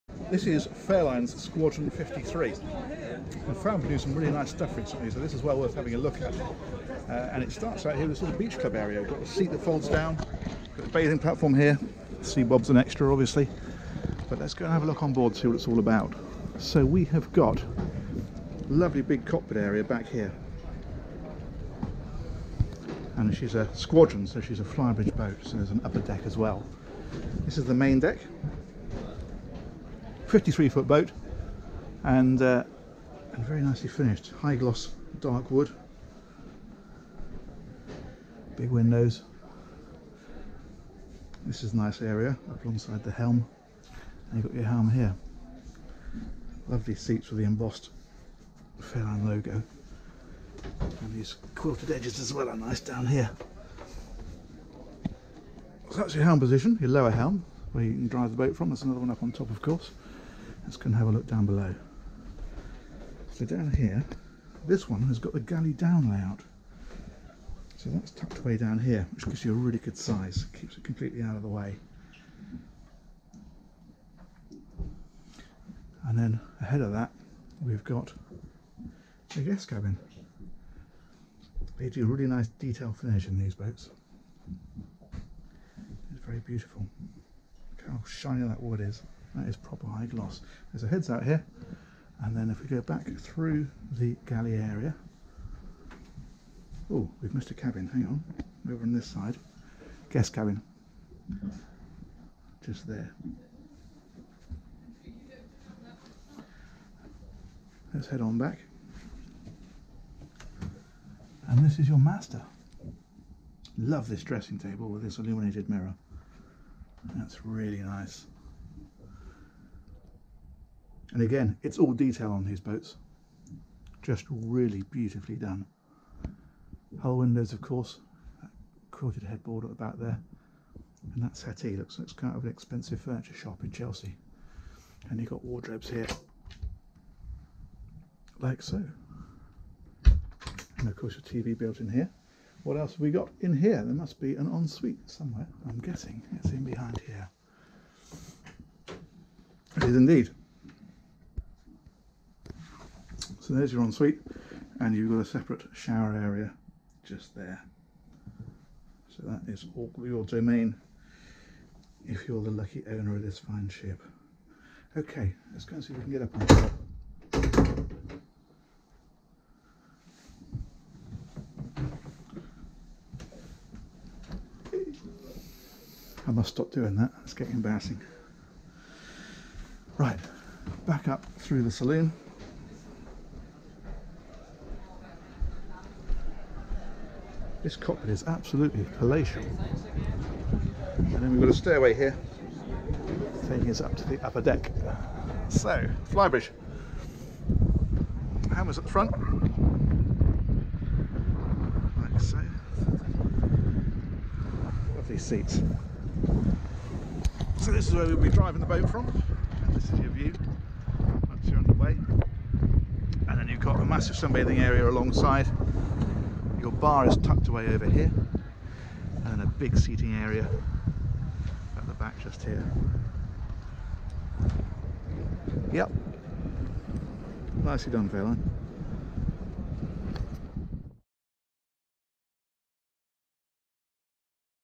Boat Show Preview : Fairline Squadron 53
The elegant and beautifully finished Squadron 53 from the British yard, Fairline.